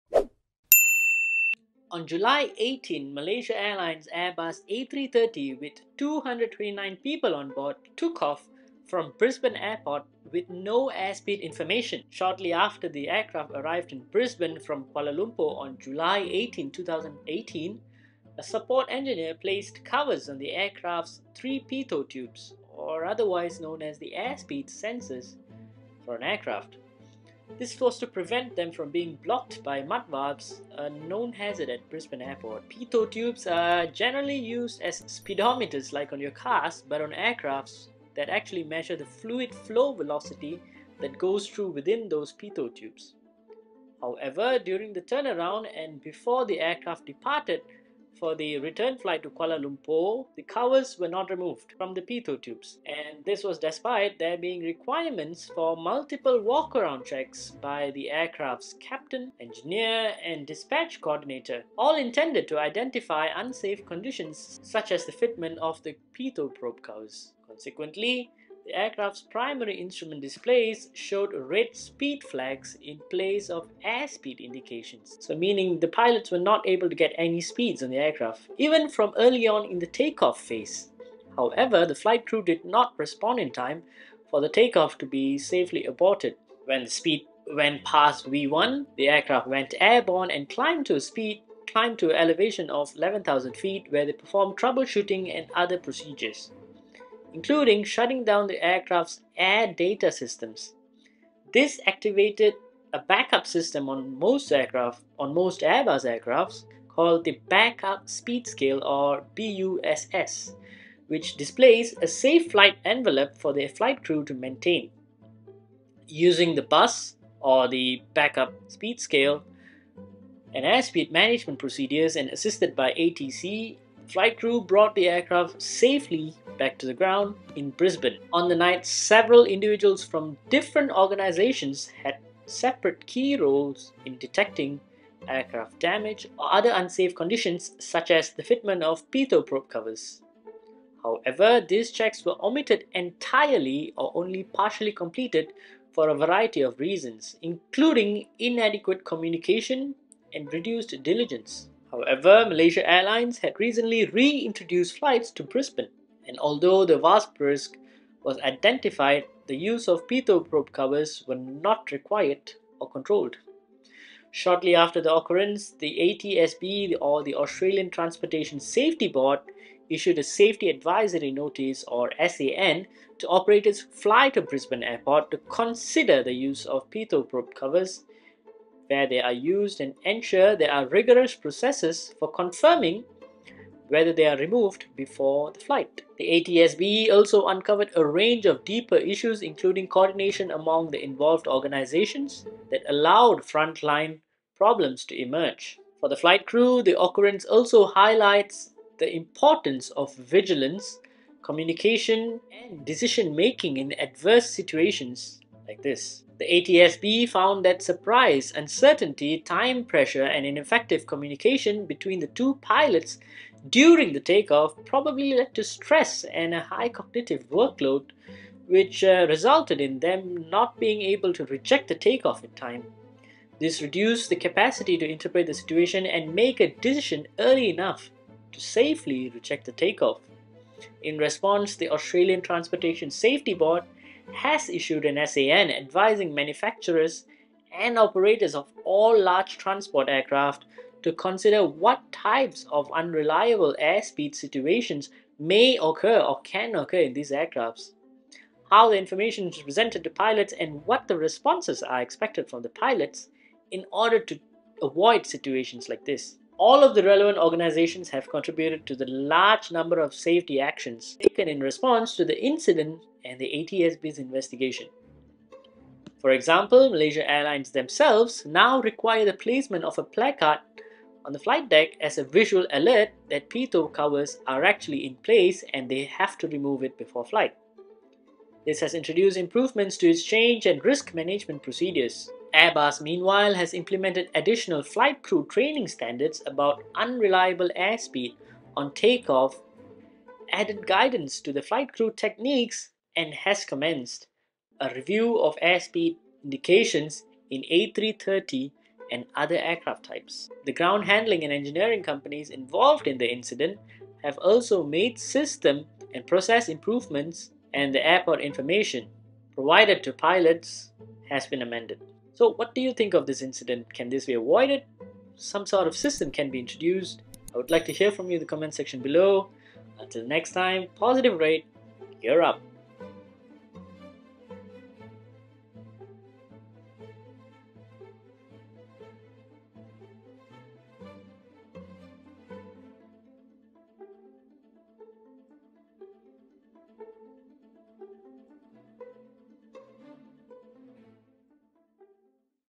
Pitot Tube Cover not Removed
On July 2018, Malaysia airlines flight 134 from Brisbane to Kuala Lumpur, flew with pitot tube cover not removed. As a result, the aircraft took off with no air speed information even from early on in the take off.

Check out this video to find out on why such a fundamental procedure was missed by several parties involved